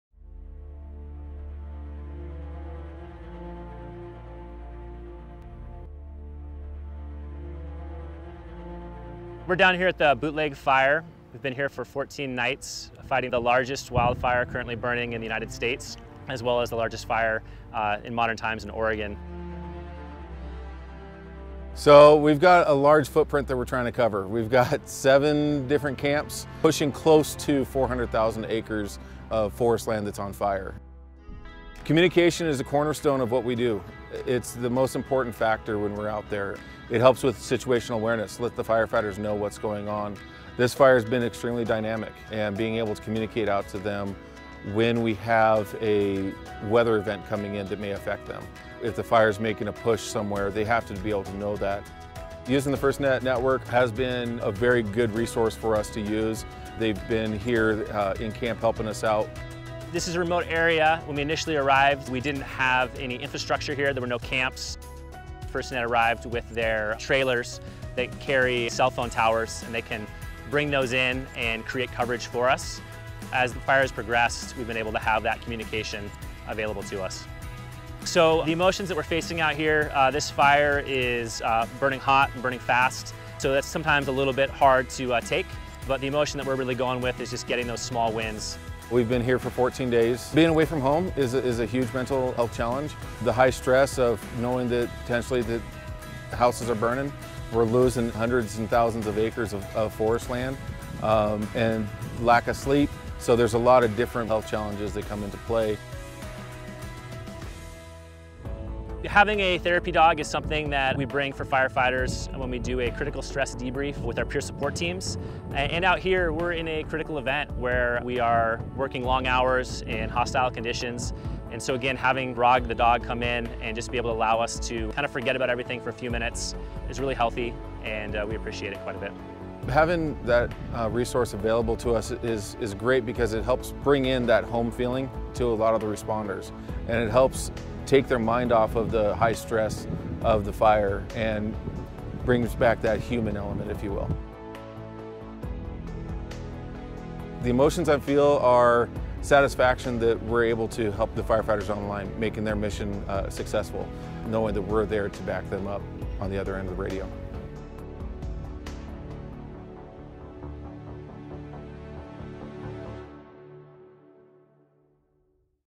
Fighting Fire: The Oregon Bootleg Fire Response
The Bootleg Fire is the largest wildfire currently burning in the United States. Here's how FirstNet, Built with AT&T is assisting fire fighters and first responders during this natural disaster.